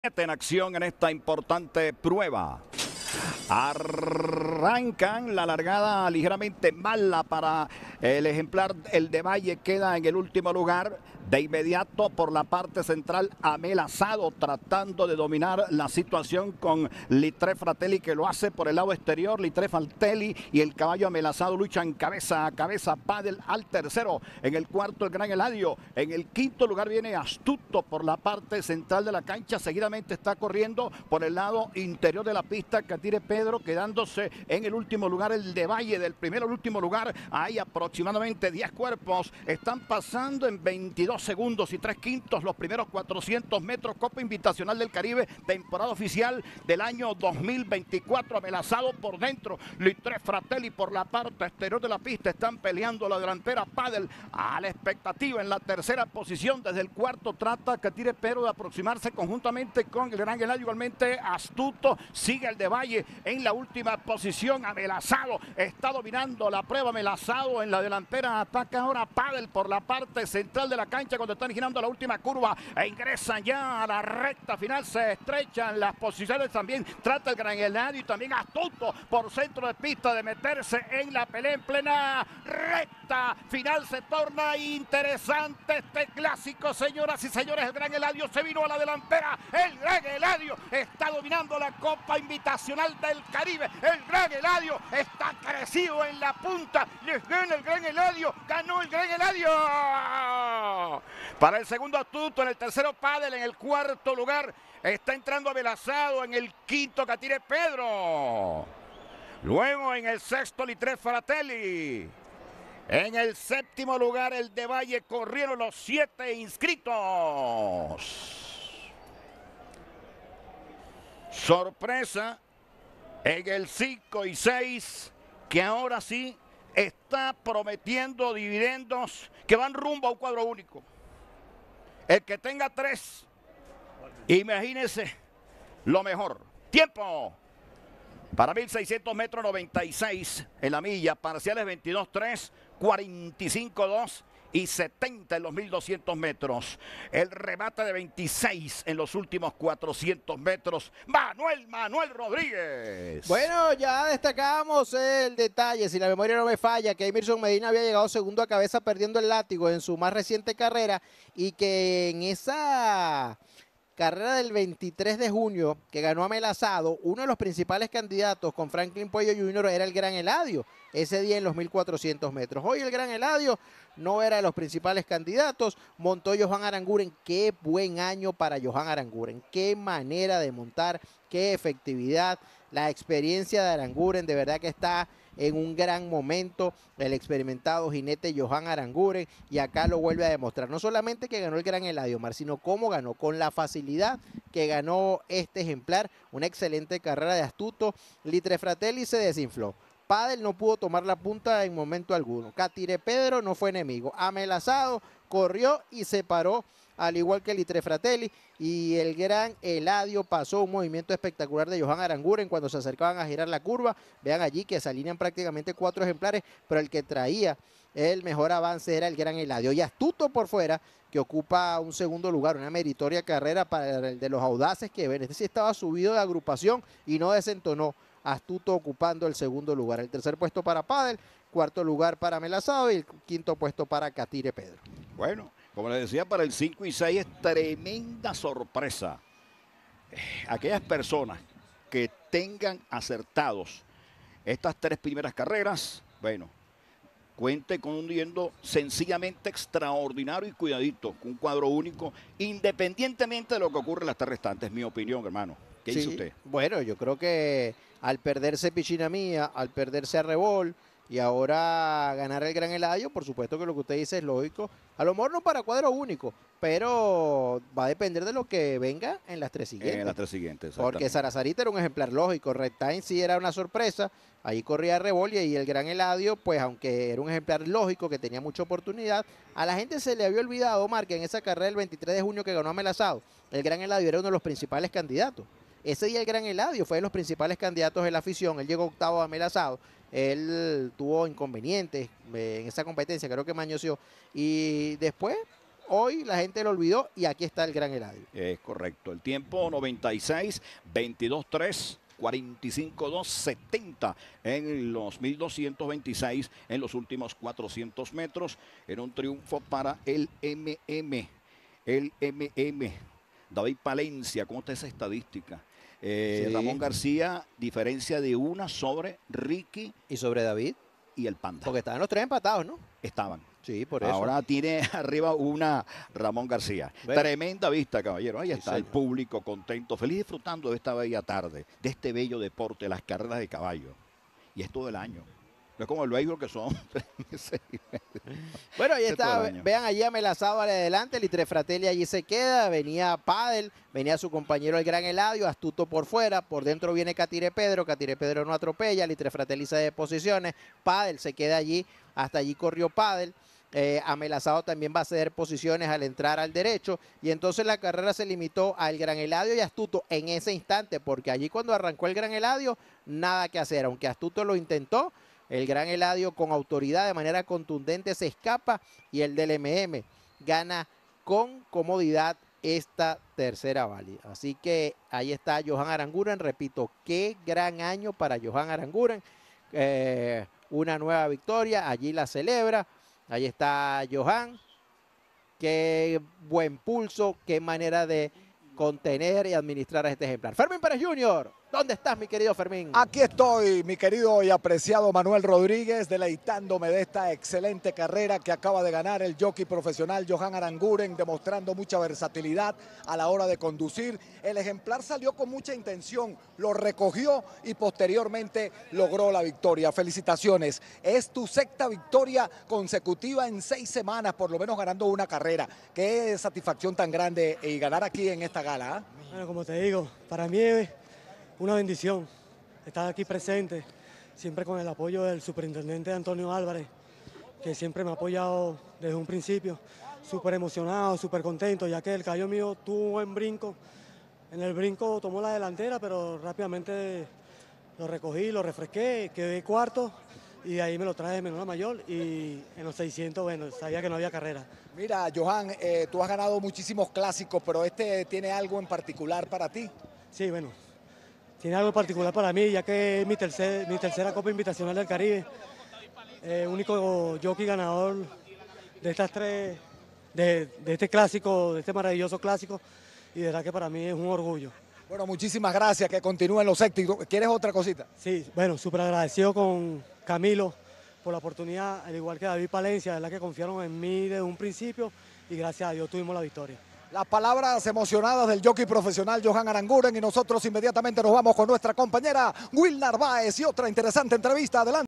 7ma. | 3era. Valida | El Gran Eladio | XIX Copa Invitacional Del Caribe | La Rinconada | Reunión 32
1: El Gran Eladio (4) | Jinete: J. Aranguren | Dividendo:  141,33
2: Astuto (3) | Jinete: F. Quevedo
3: Li Tre Fratelli (7) | Jinete: M. Rodriguez
4: Amelazado (5) | Jinete: H. Medina
5: Catire Pedro (2) | Jinete: R. Capriles
Distancia: 1600